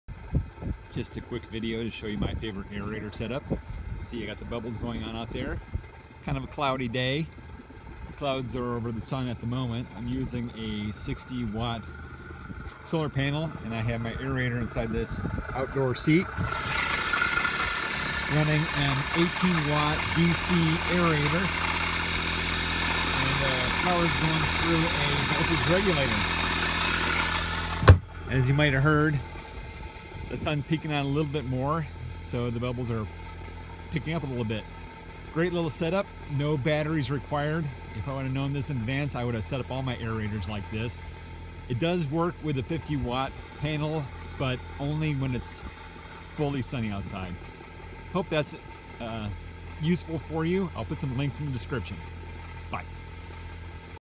Solar Pond Aerator no inverter or battery
This is my favorite aerator setup. I have a setup using batteries and a low voltage cutoff, but this setup is WAY cleaner and cheaper. The DC aerators are about $25 and last me about 2 seasons on average.

Voltage regulator from Amazon
18W DC Aerator from Aliexpress (if you shop around on this site, you can probably find it a few dollars cheaper)
(this one seems a little more reliable)
Solar panel is no longer available, it was an ACOPOWER 60W Poly panel, I'd think any 60 Watt solar panel would work fine.